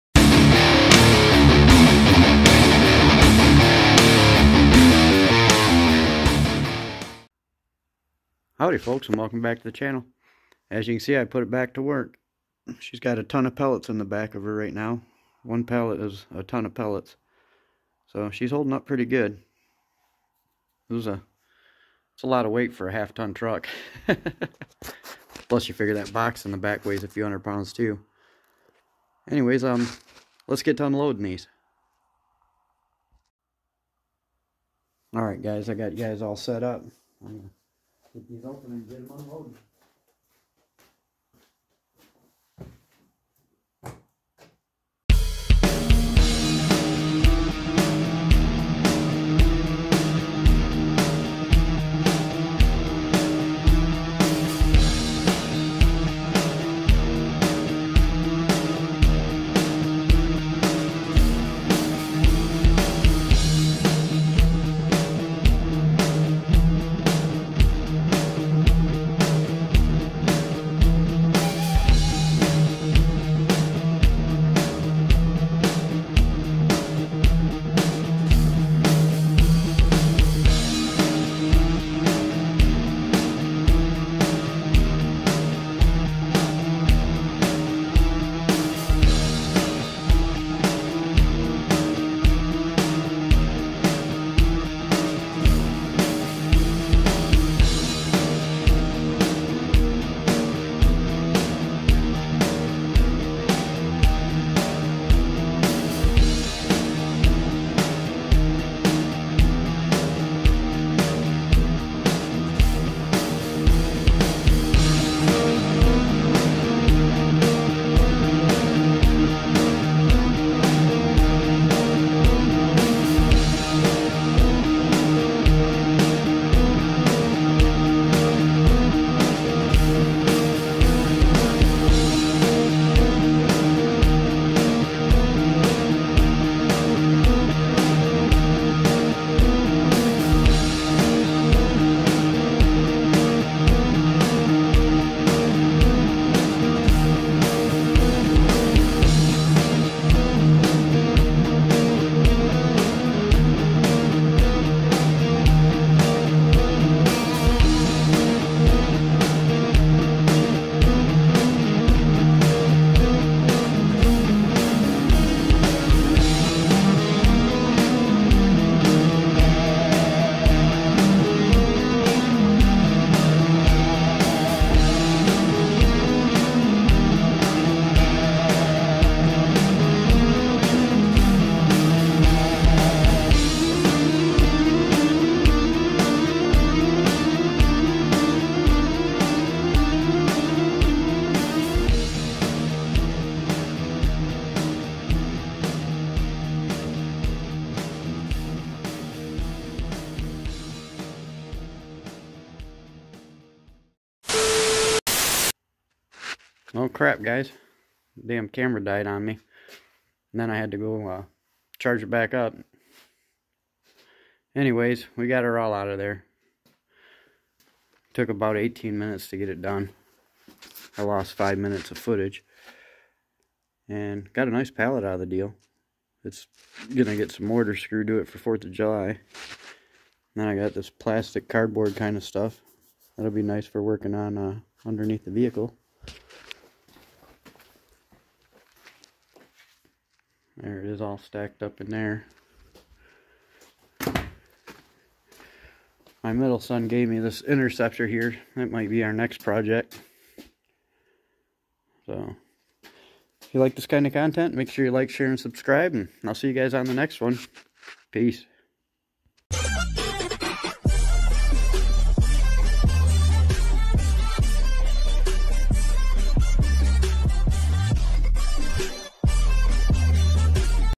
Putting Myself & The Old Dodge Ram To Work!!!
Howdy folks and welcome to the channel. Just a quick video of putting the old truck back to work. Had around 2400 pounds in the bed.

Intro Audio track and music in this video by:

 "JAECize of Unreal Anomaly Productions" on YouTube